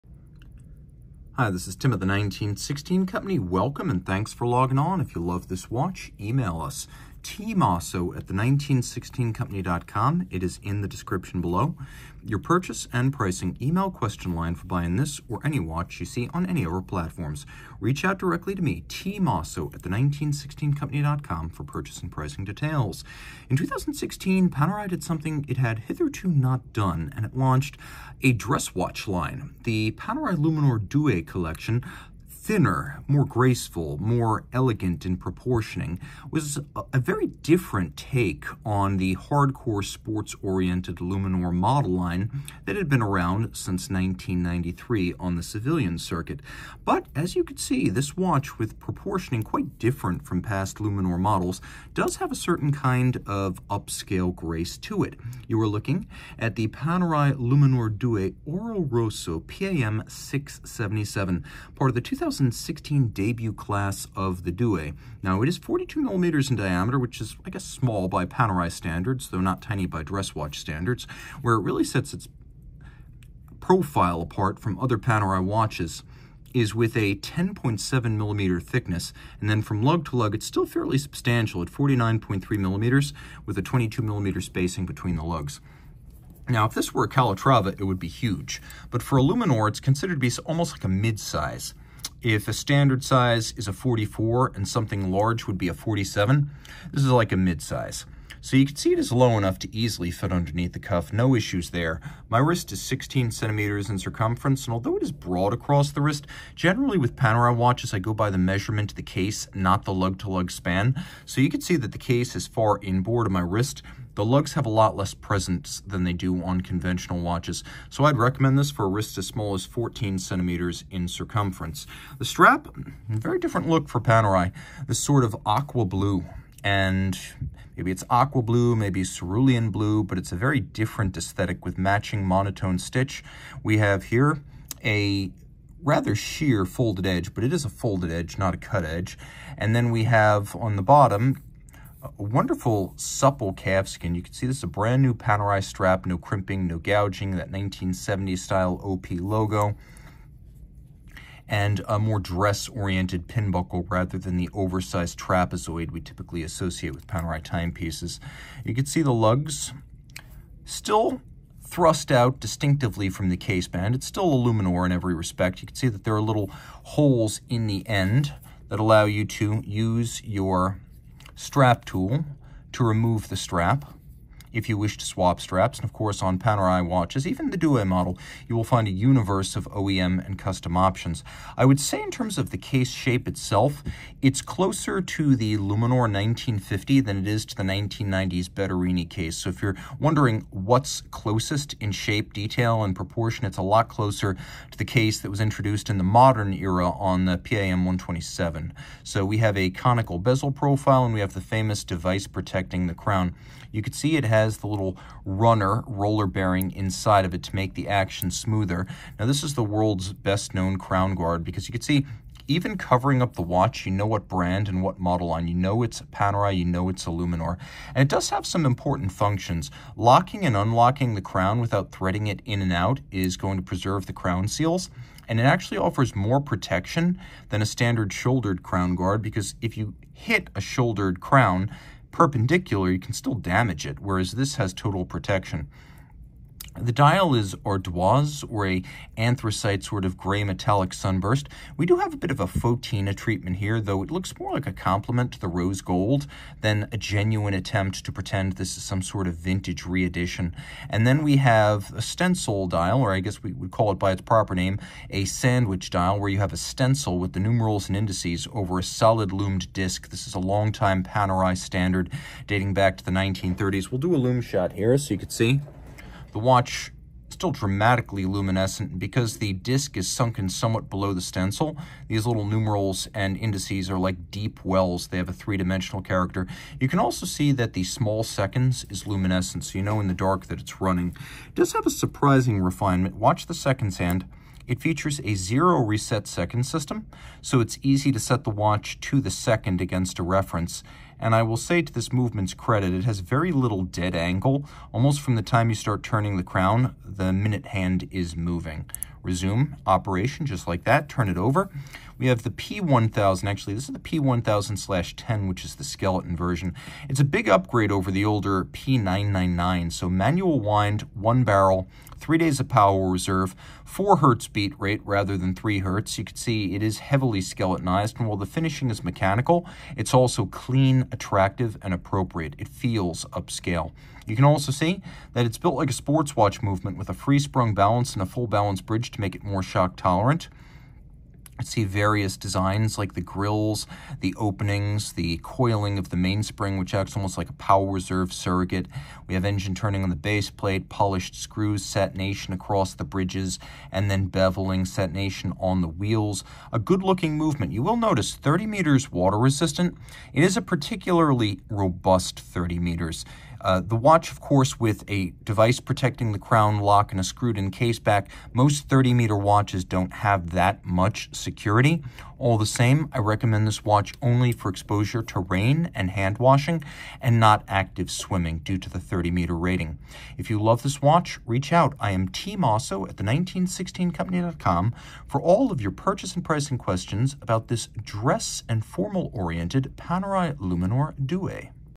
Panerai Luminor Due (PAM00677) Panerai Watch Review
SKU: 4980199

The Panerai Luminor Due (PAM00677) is encased in 42mm of 18k rose gold surrounding a gray anthracite sun-brushed dial on a blue alligator strap with an 18k rose gold tang buckle. Features of this Panerai Luminor Due include hours, minutes and small seconds.

For complete details, watch the full Panerai Luminor Due review.